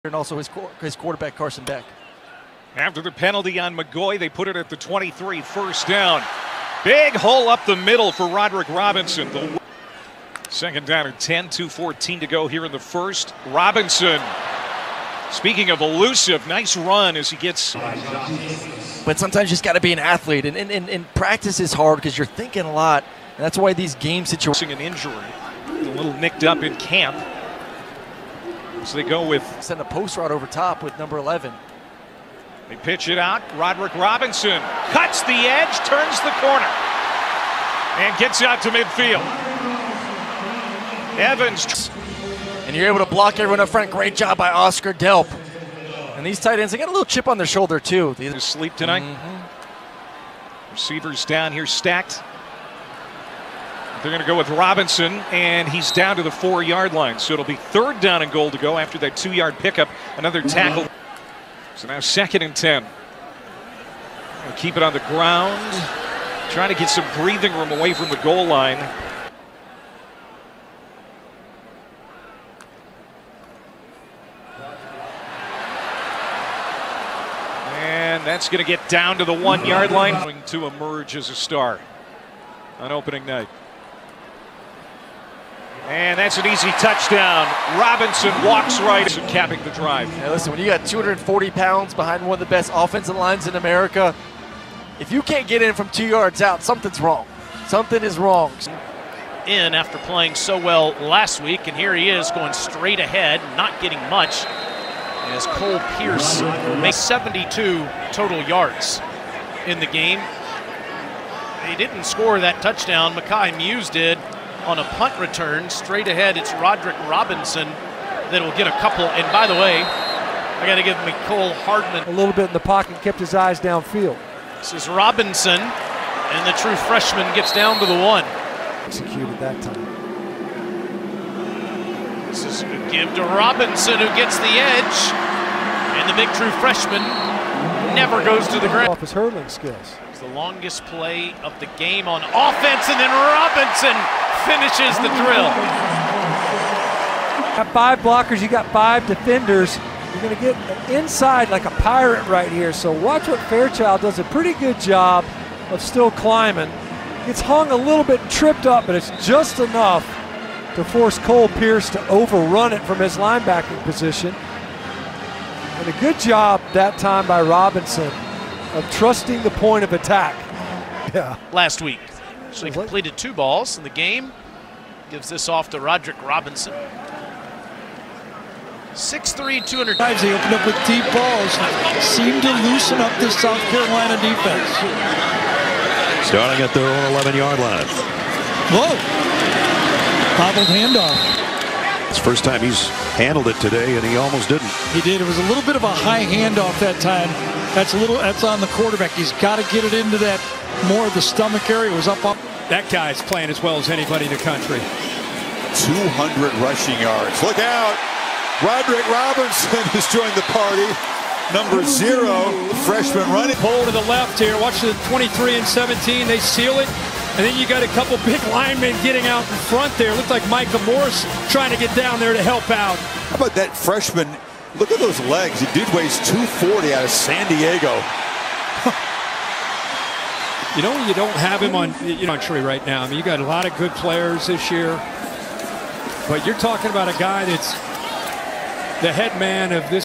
Rodrick Robinson 2023 Film - All Carries
All of Roderick Robinson's carries from the 2023 season.

Clips from the Orange Bowl vs Florida State were omitted because of copyright.

Stats:
24 carries for 196 yards, 2 tds